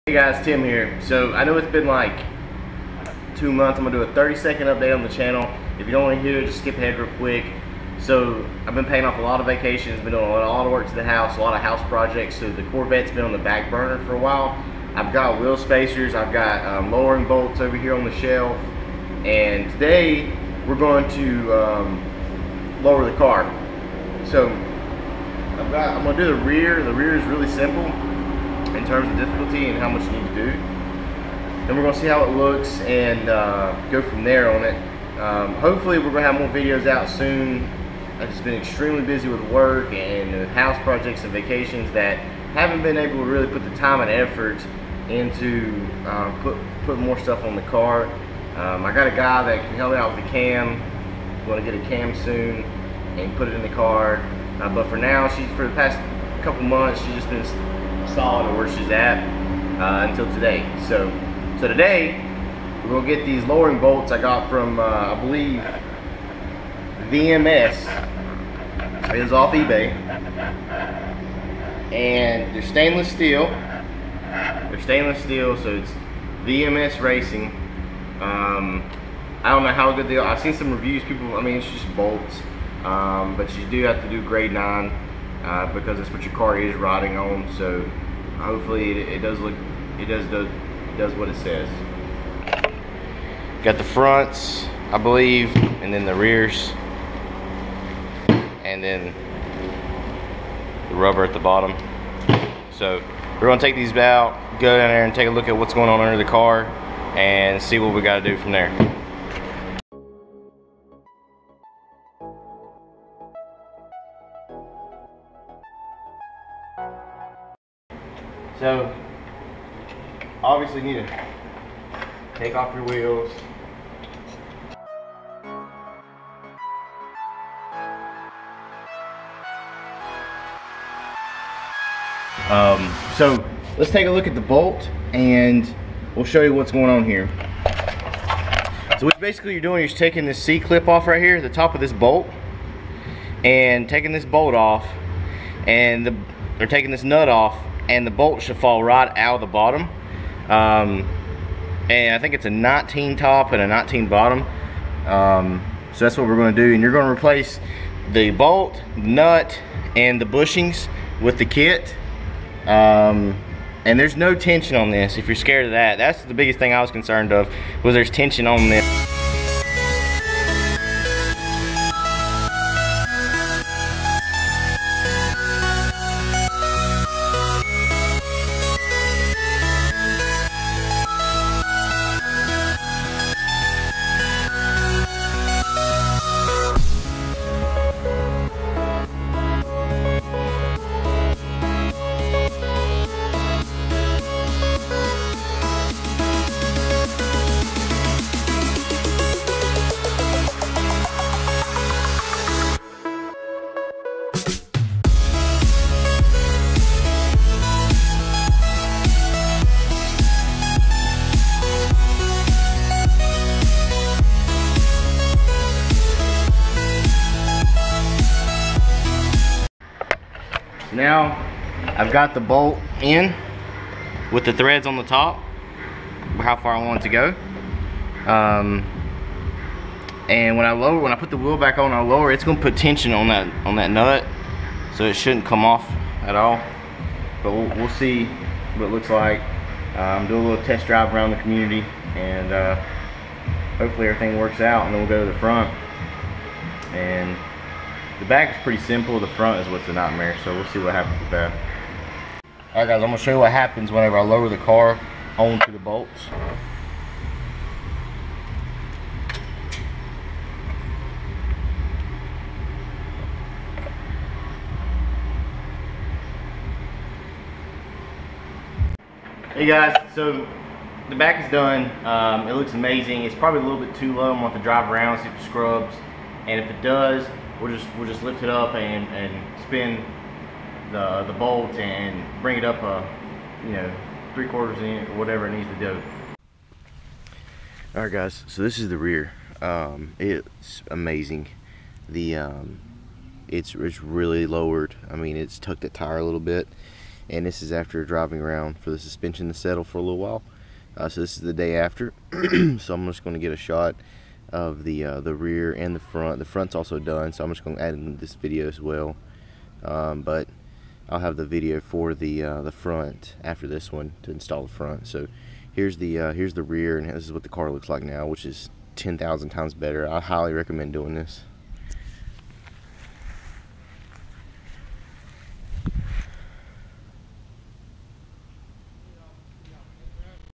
C5 Corvette REAR lowering bolts install!
Hey guys, new lowering bolts install on the rear! Fronts coming out tomorrow.